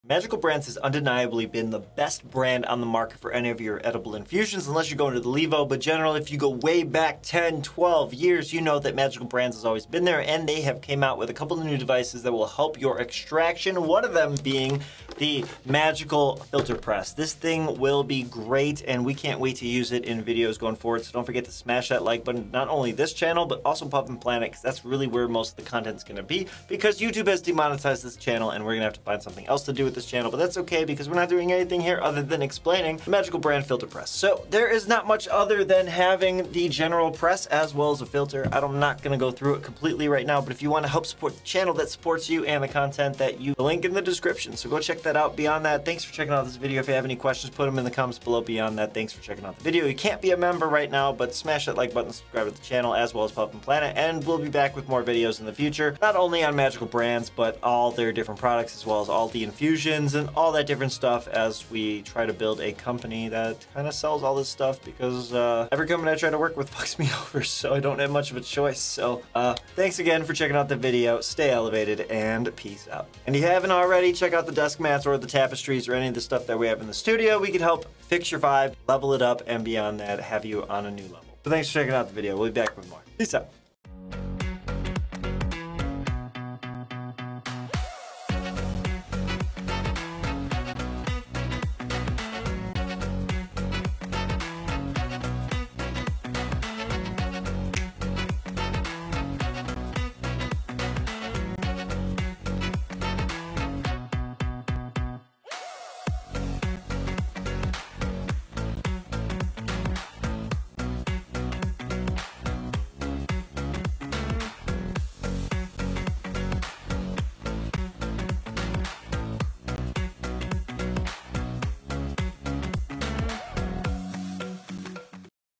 The Magical Filter Press - Mess-Free Extraction for Mason Jars
Magical Filter Press -The Ultimate Mess-Free Plant Matter Removal System for Butter or Oil Extraction- Fits Mason Jars with Plunger and Cleaning Brush for Easy Maintenance and Cleaning

Channel Merch

OG Peak & Pro CCV Whip Attachment is Back! Overview & Function Testing OG & Pro! 1 Terp Test

Puffco Peak Pro Video List